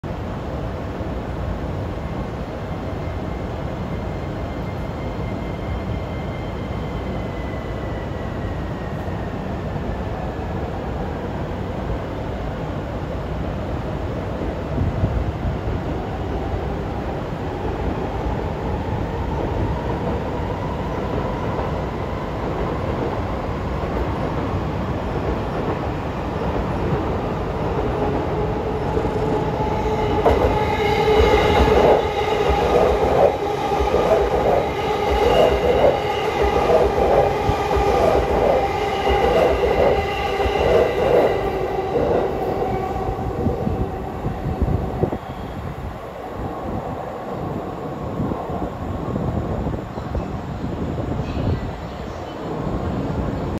【E233系H007編成 TK入場回送（指定保全）】E233系6000番台ｸﾗH007編成が回送電車として横浜駅10番線を通過するシーン（回8840M）2024.3.18
久里浜ウヤったので同時刻に横浜駅を通過するこちらを撮影
ダイヤ乱れの影響で大船駅での発着順次変更を受け、少し遅れての通過となりました

横須賀線のホームを横浜線の車両が通過は違和感ですなぁ、

晴れていましたがどっかの誰かさんとは違うのでちゃんと幕は止めております👍
同業者はいませんでした。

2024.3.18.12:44